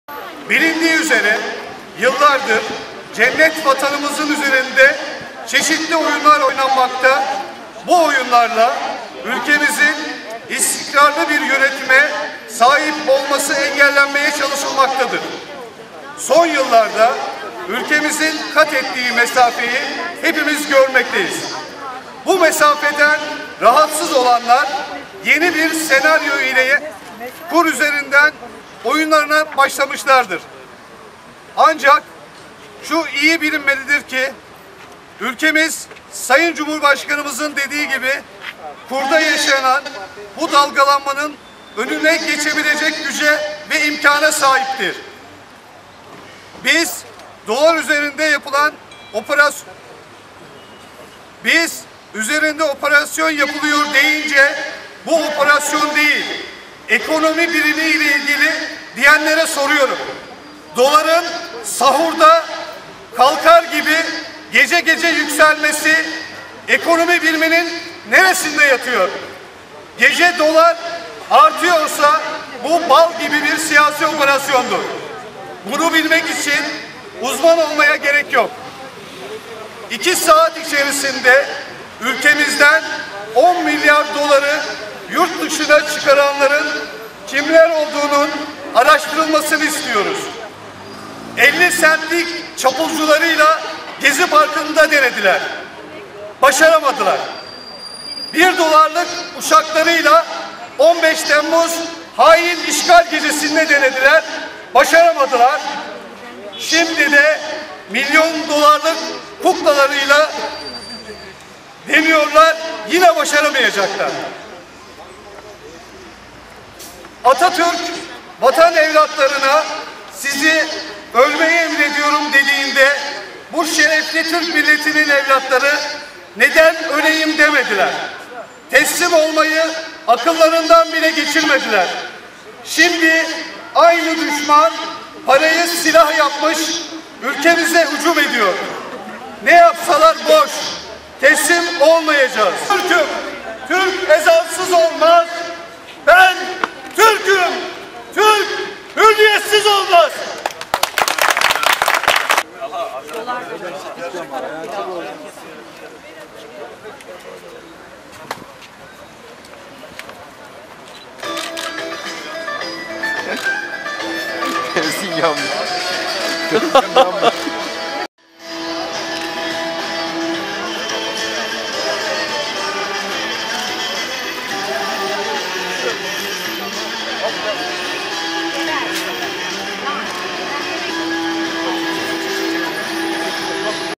DOLARI YAKARAK PROTESTO ETTILER
Sinop MİDDER İl Başkanlığı son günlerde doların yükselmesi sebebiyle protesto düzenlendi.